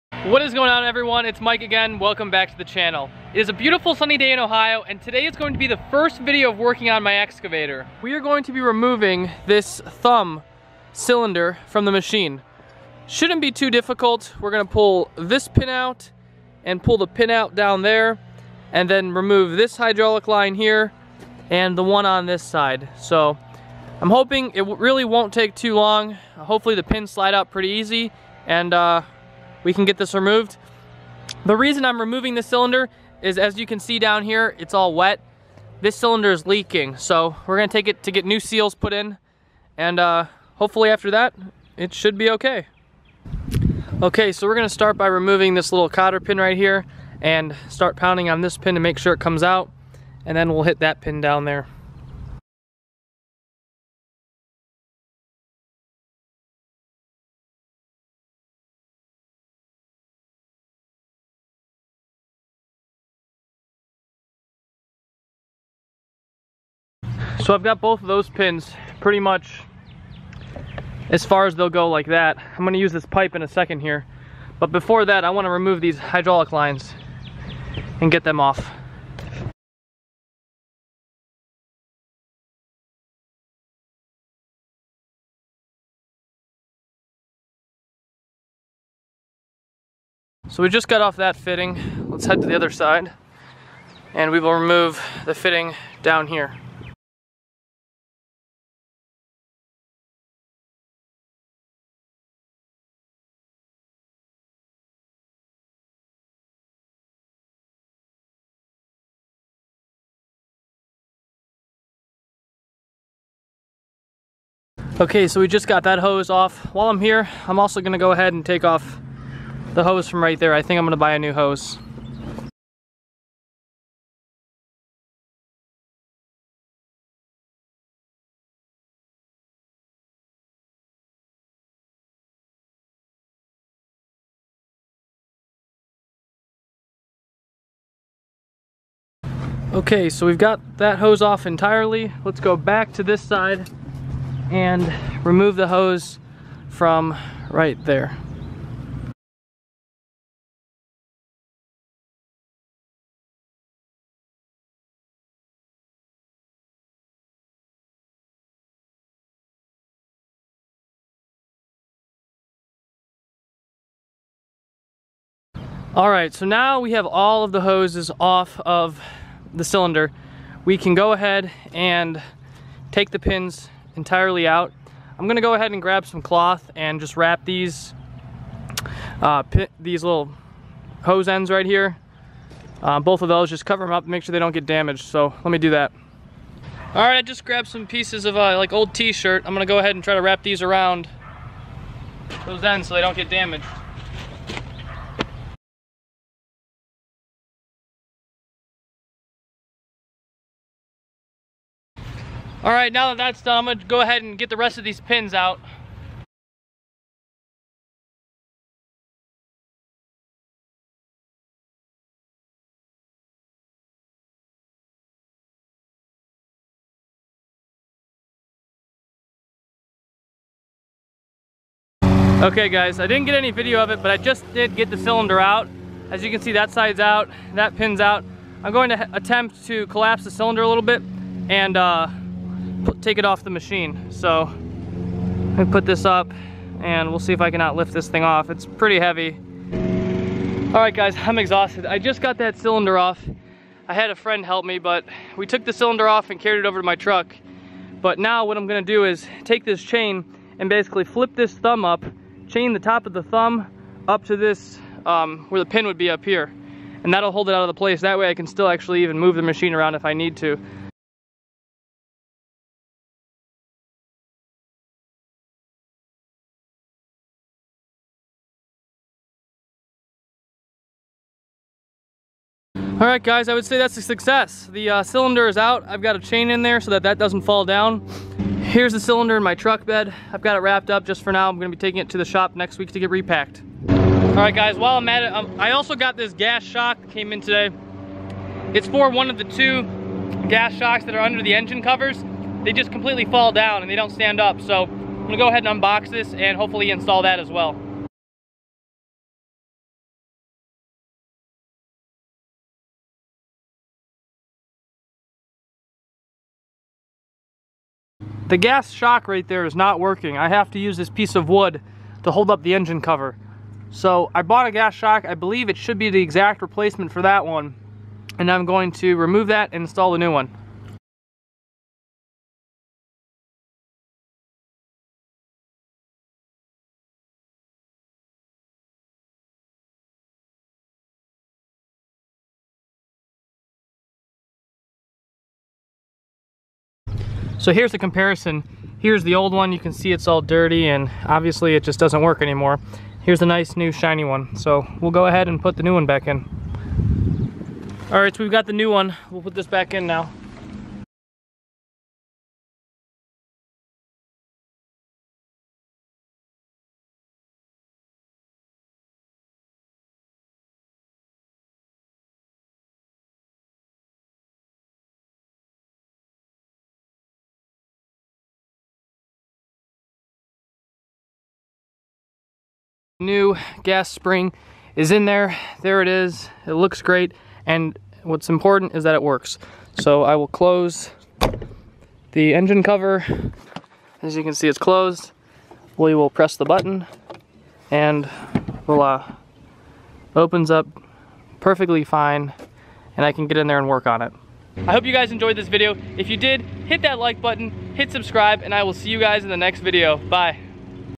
REMOVING AN EXCAVATOR THUMB CYLINDER
Today, I remove the thumb cylinder on my 2003 Bobcat 442 Midi excavator.  The cylinder was badly leaking, and I am taking it to a shop to be rebuilt.  I will be posting more videos with fixing, and general maintenance on the machine.

Links to parts used in this video:
Action Camera
Craftsman Tool Set
New Holland Lanyard
Seat Cover
LED Work Lights
4 1/2" Angle Grinder